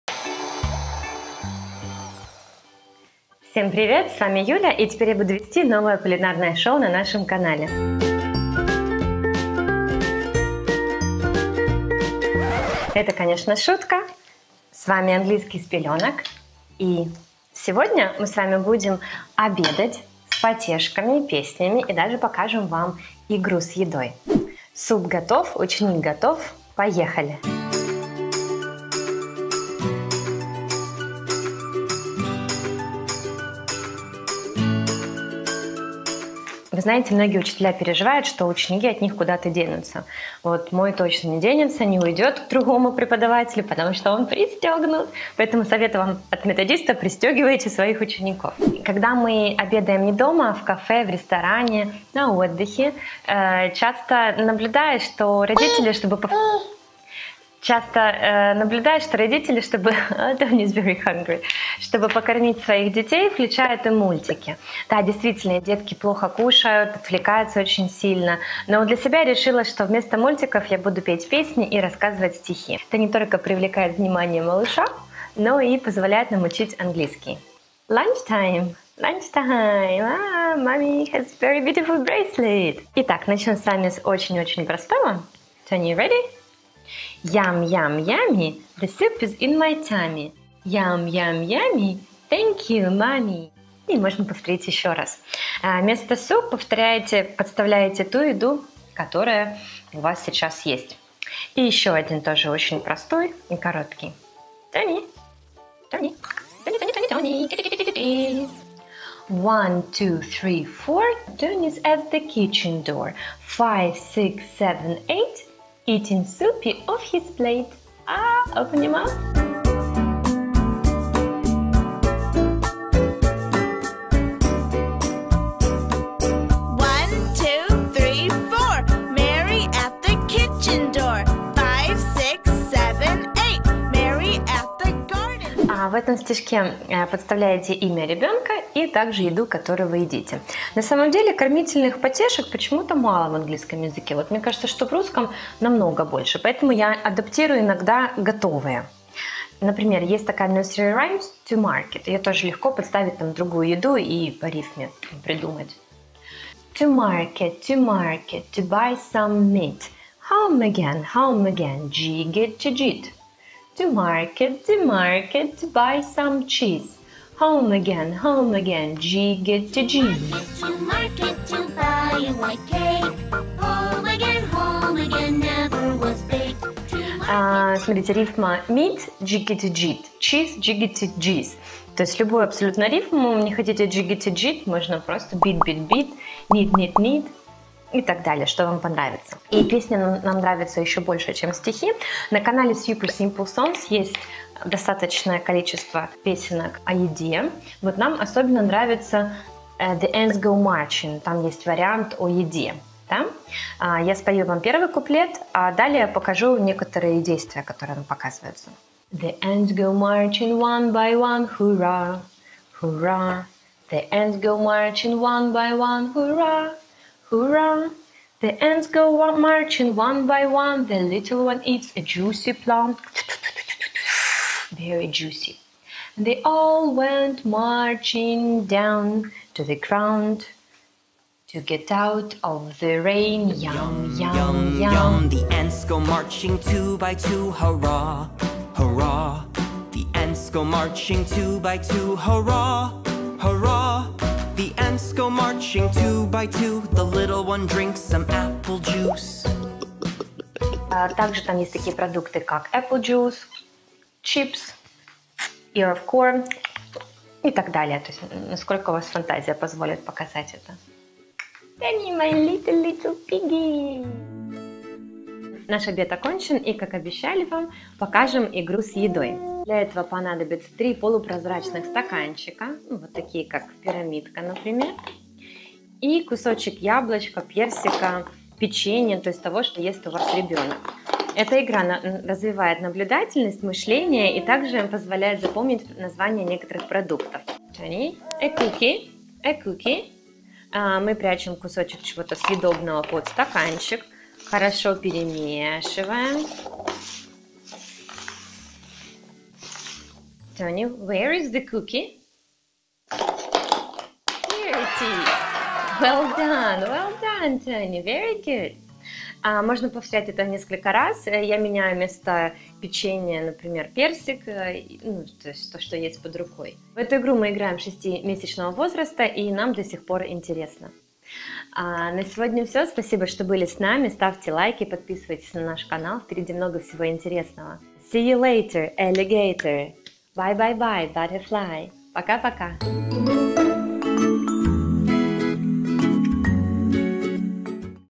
ГОТОВИМ с МАЛЫШОМ. ПЕСНИ и ИГРЫ на АНГЛИЙСКОМ с РЕБЕНКОМ I Teaching Kids I Выпуск #4 I Skyteach
Вы легко привлечете внимание малыша, чтобы накормить. А заодно выучите названия некоторых продуктов. Малыши с удовольствием повторяют за мамой. Обучение английскому языку станет вашей веселой игрой.
В этом видео вы найдете примеры потешек и песенок, которые сможете использовать и адаптировать для себя.
А еще мы покажем вам игру с едой, которая понравится вашему малышу. Пусть ваши детки едят с аппетитом!